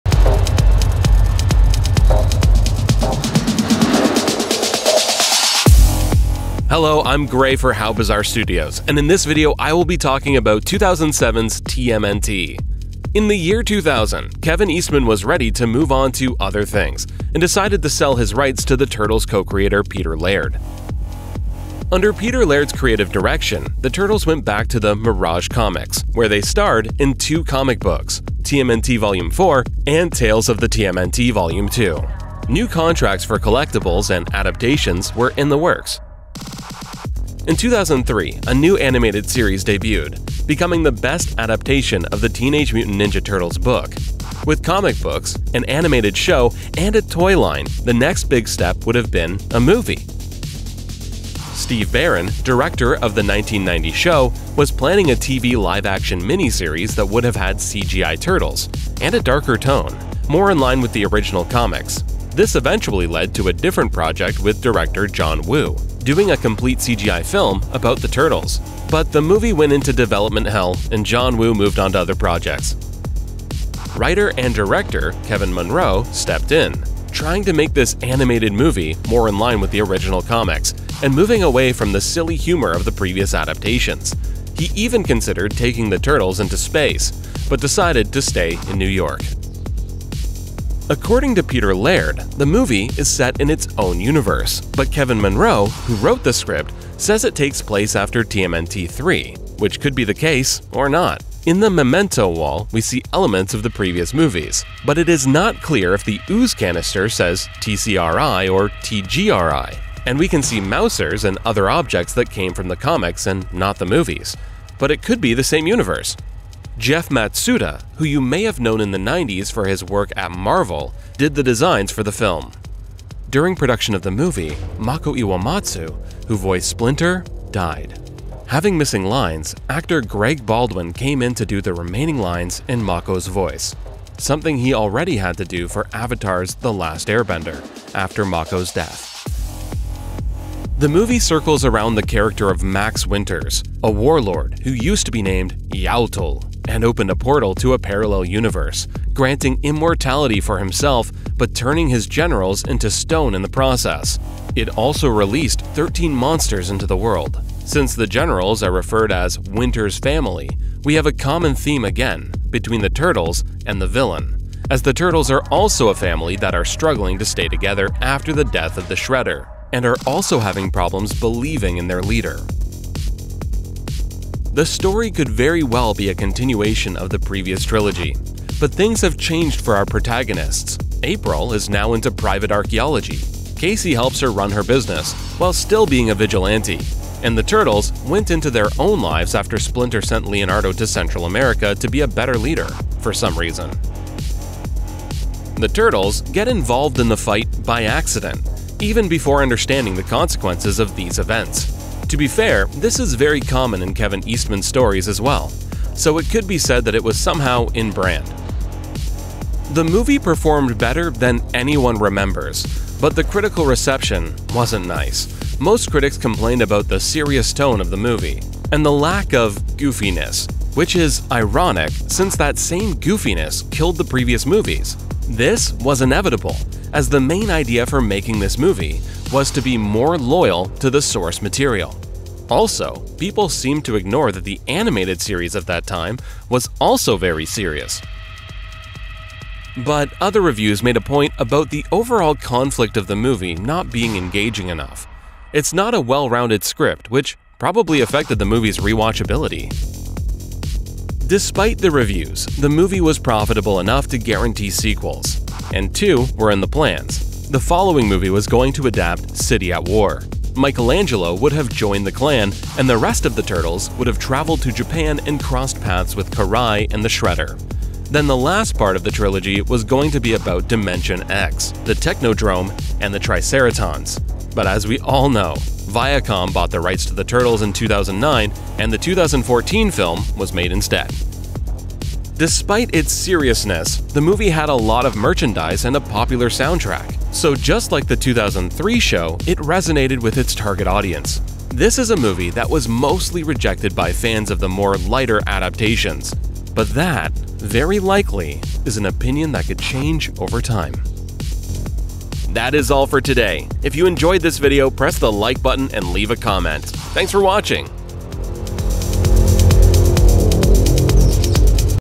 An underrated gem - TMNT 2007
In this TMNT movie review, the 2007 CG animated film that tried to be loyal to the tone of the comic-books, but wasn't received well by mainstream audiences.

Javier S. Montano

🎙️NARRATED BY
Grey Beard Wizard

🔖 An underrated gem - TMNT 2007 - CHAPTERS
00:00 The road to TMNT 2007
01:13 Kevin Munroe comes in
02:20 Plot
03:29 Critical reception
04:10 Planned sequels
04:42 Popularity